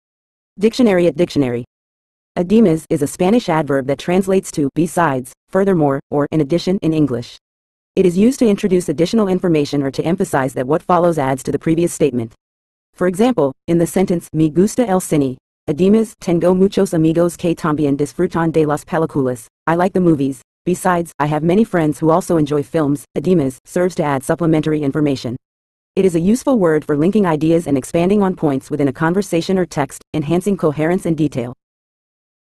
además Meaning In English
"Además" is a Spanish adverb that translates to "besides," "furthermore," or "in addition" in English. It is used to introduce additional information or to emphasize that what follows adds to the previous statement. For example, in the sentence "Me gusta el cine; además, tengo muchos amigos que también disfrutan de las películas" (I like the movies; besides, I have many friends who also enjoy films), "además" serves to add supplementary information. It is a useful word for linking ideas and expanding on points within a conversation or text, enhancing coherence and detail.